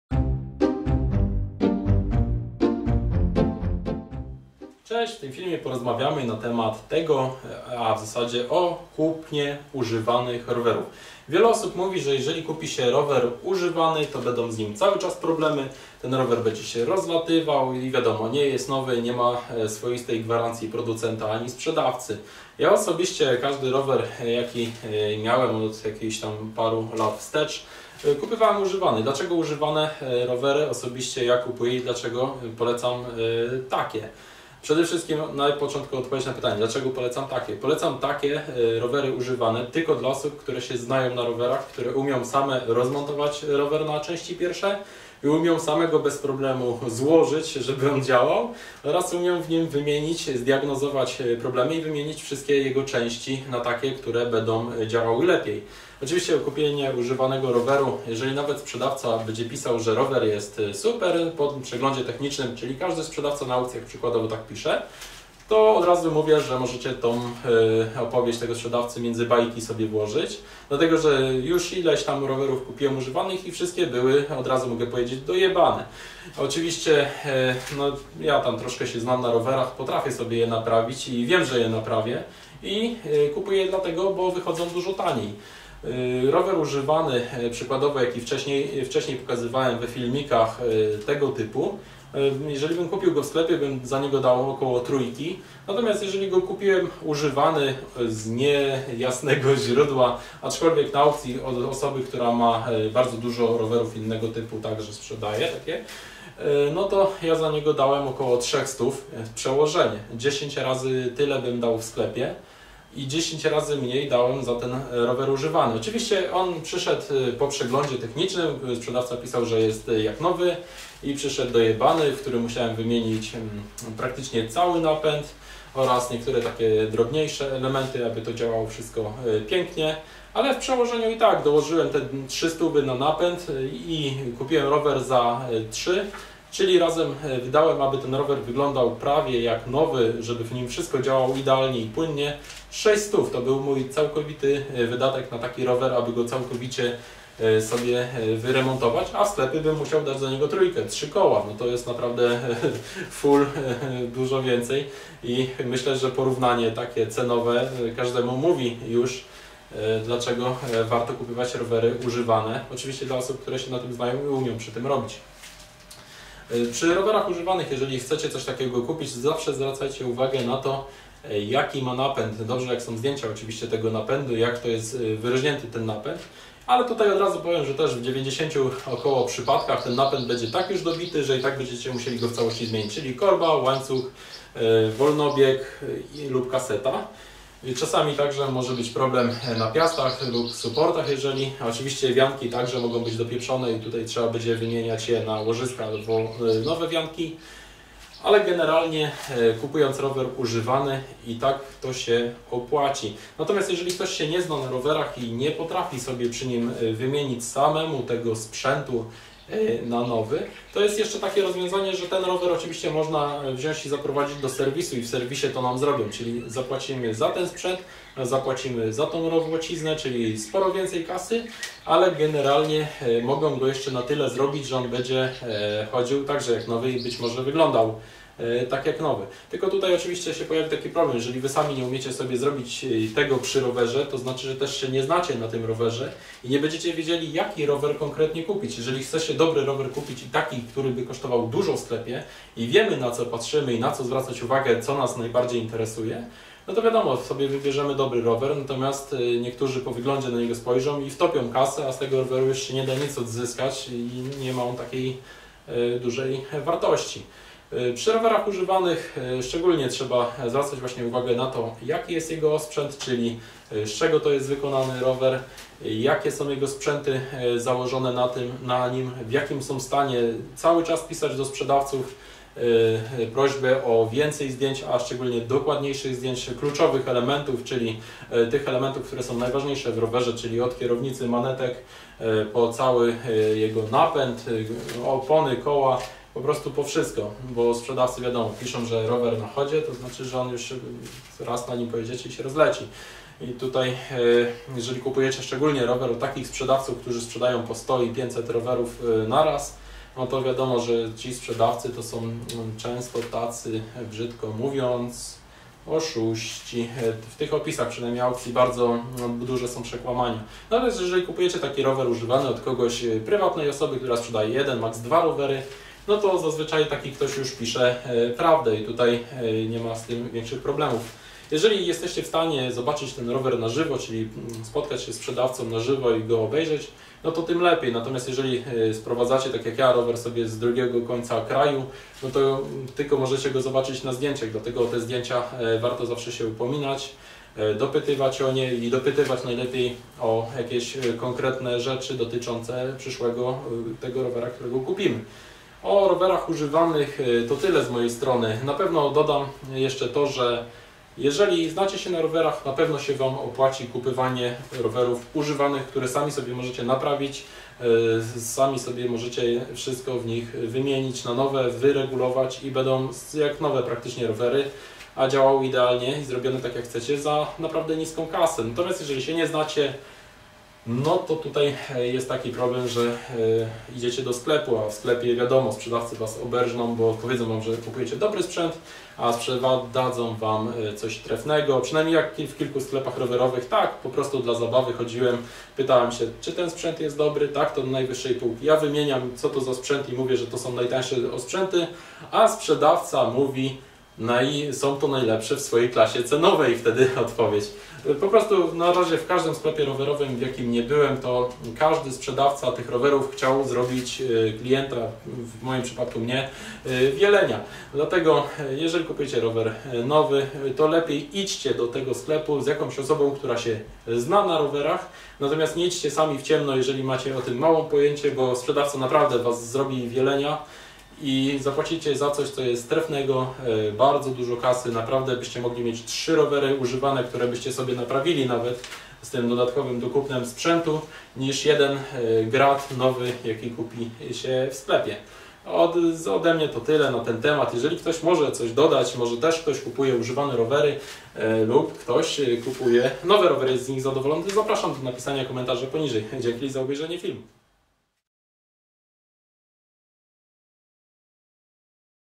Rowery używane - czy warto kupić używany rower
Wiele osób się zastanawia czy warto kupować roweru używane. Odpowiedź jest prosta  -zdecydowanie warto, ale trzeba się na nich dobrze znać. Używany rower może mieć problemy techniczne - jeśli się je umie naprawiać to można nabyć super rower za bardzo niską cenę - nawet 10 razy taniej niż w sklepie.